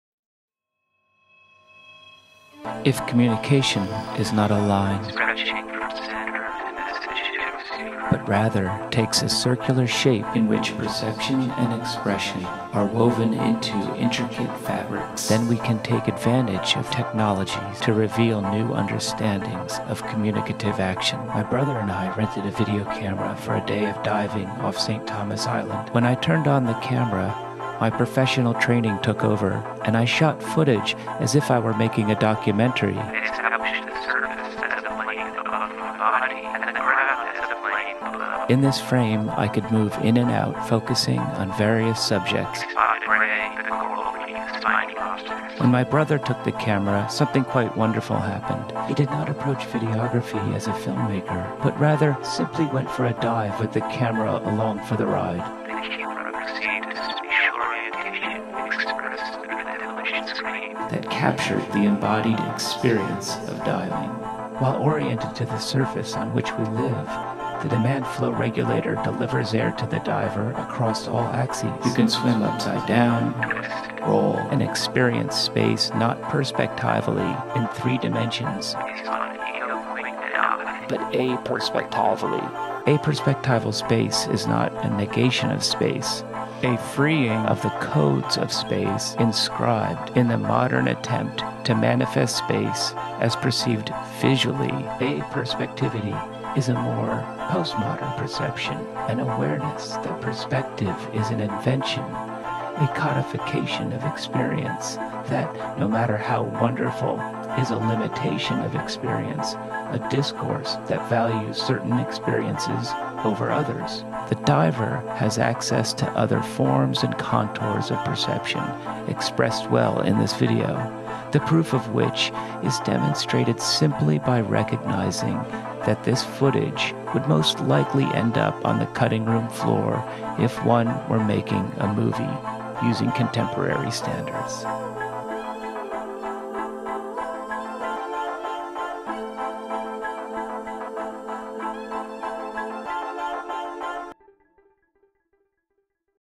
Upside Down
Underwater space, as perceived by scuba divers', breaks three dimensional and four dimensional discourses of space-time experience.  (Please note that communicology accepts physics as an outstanding way of measuring the physical world; our experience of that world will, however, vary).

This video, "upside down" is an excerpt from my book, The Depths of Perception:  A Communicology of Scuba Diving, available for free at the iTunes book store (the text is currently available as a promotional except).

The video introduces the Jean Gebser's theory of aperspectival space (citation in text).  Aperspectival space reveals the limitations of three dimensions (hence "perspective"), and embraces a time-space experience closer to Picasso's and Einstein's work, but with differences discussed in the course text.